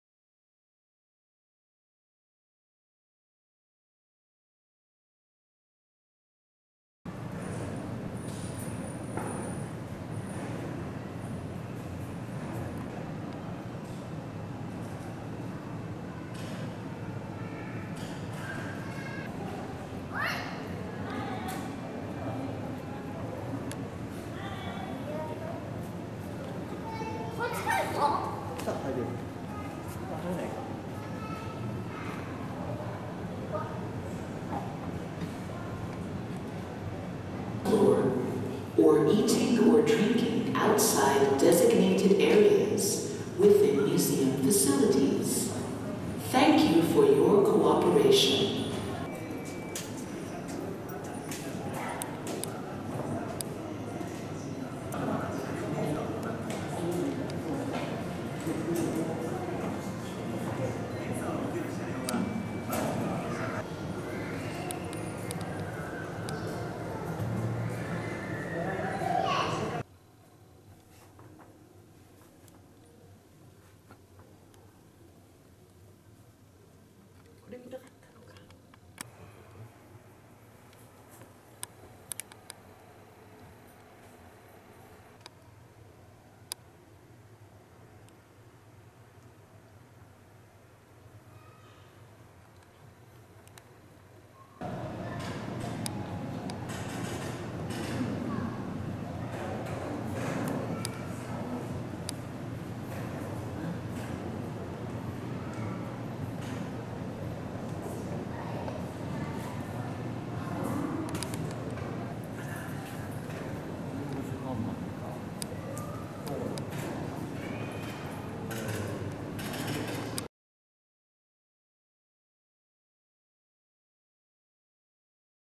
京都鉄道博物館 サロンカー「なにわ」展示 2019-5-17
京都鉄道博物館で展示されたサロンカー「なにわ」を撮影したものです。